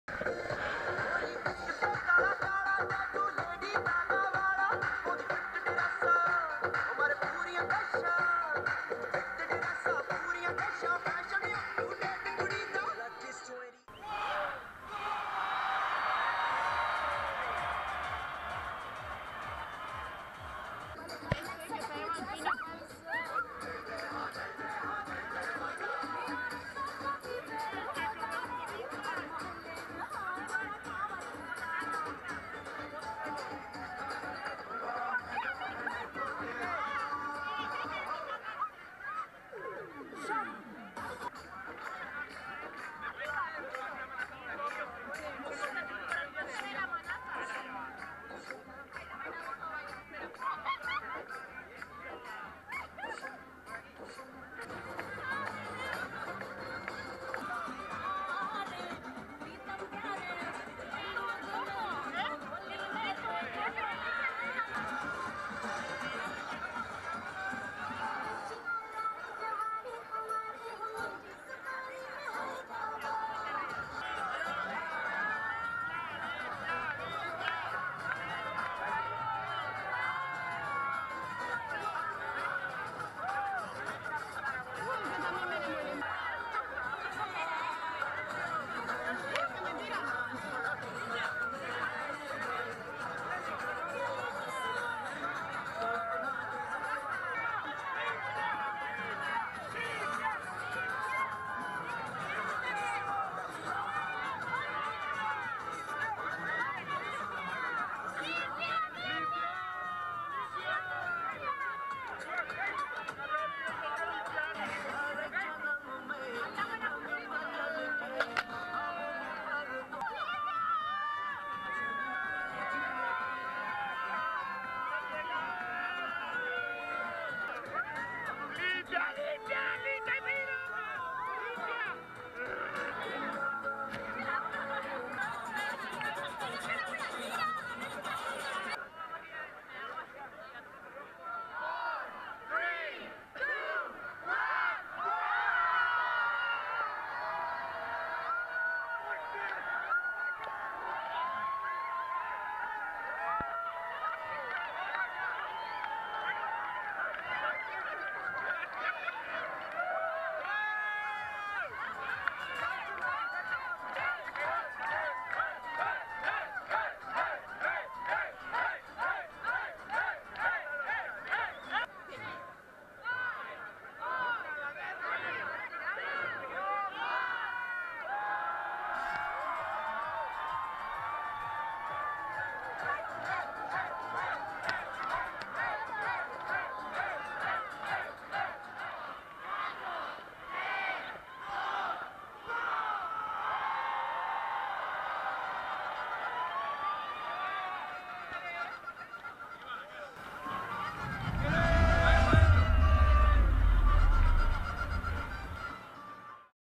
Holi Monsoon Madrid 2015 – SJCAM SJ400 WiFi – Flygia360
This was the atmosphere at the Holi Monsoon party in August 2015, in Madrid (Spain). Colors everywhere, Bollywood dancing and so much fun!
Filmed with my SJCAM SJ4000 WiFi action camera